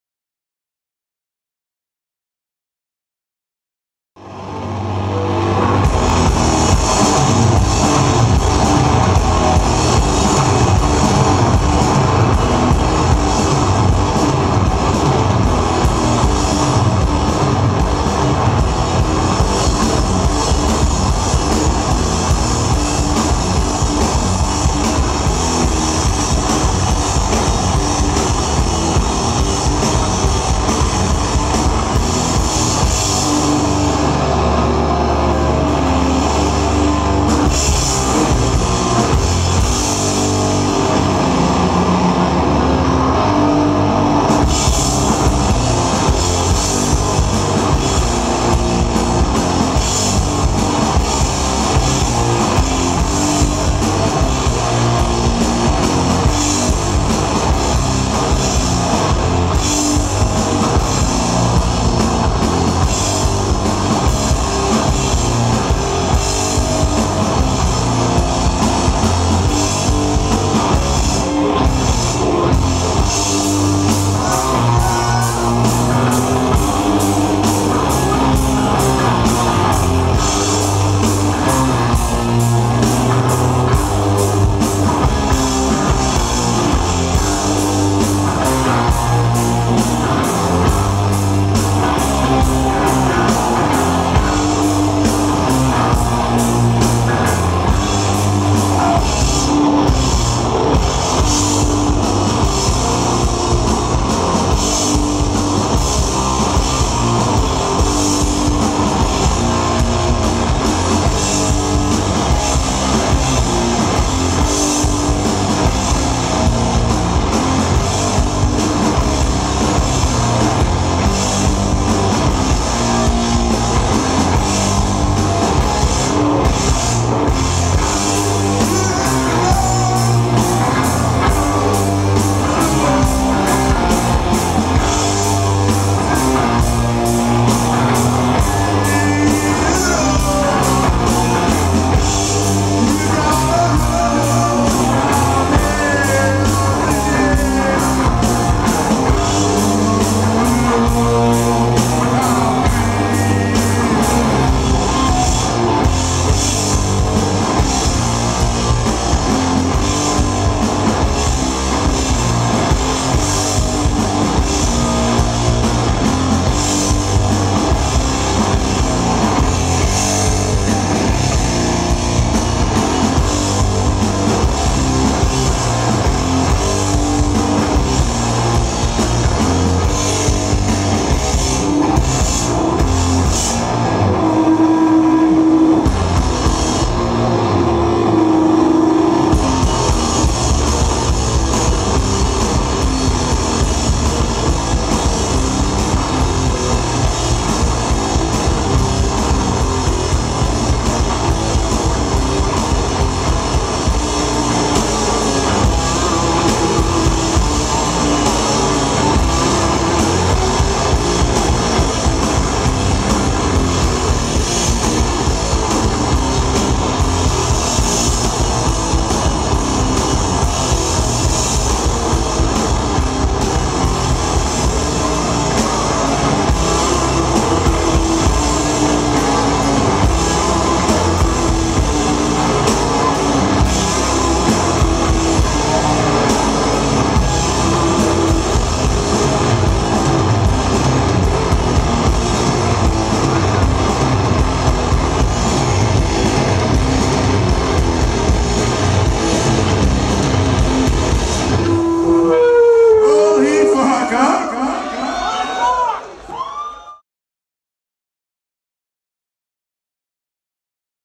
Manlord -5.mp4
Manlord Live at The Oxygen Complex Moncton,April 30th 2010-Opening for Subliminal CD Release